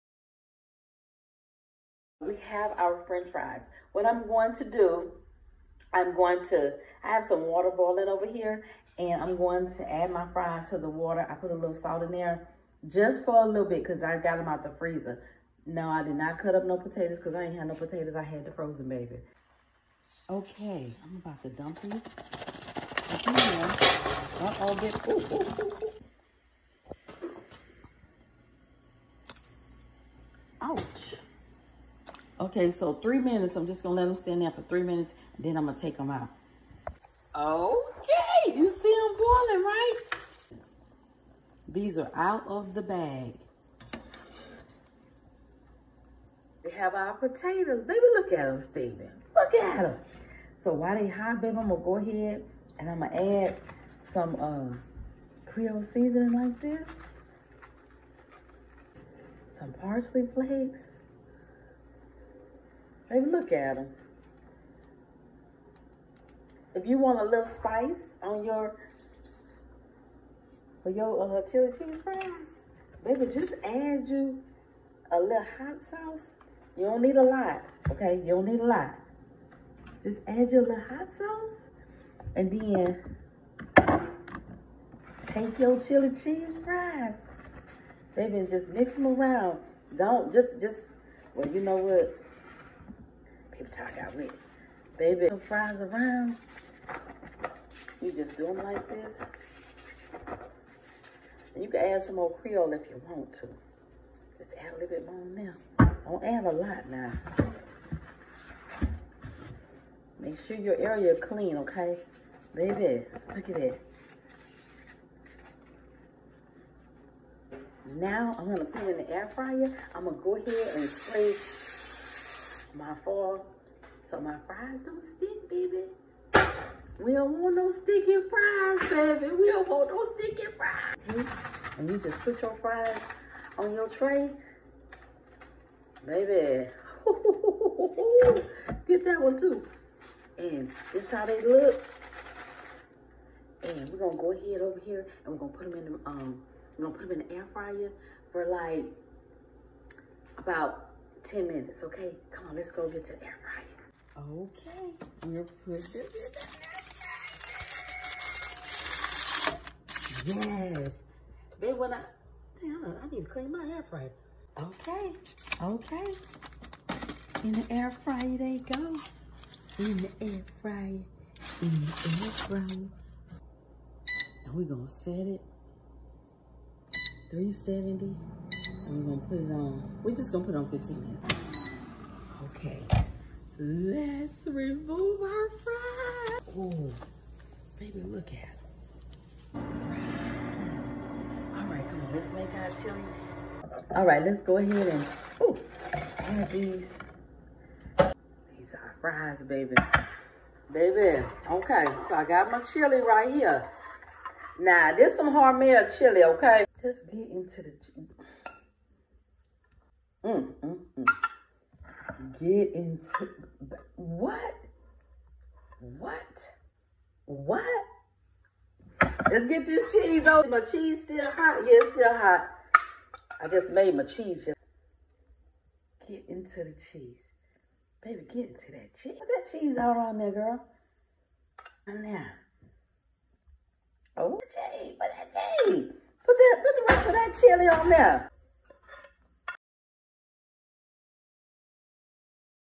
Chili Cheese Fry Lovers Will Go Absolutely Crazy For This Recipe!
this video is about chili cheese fries at home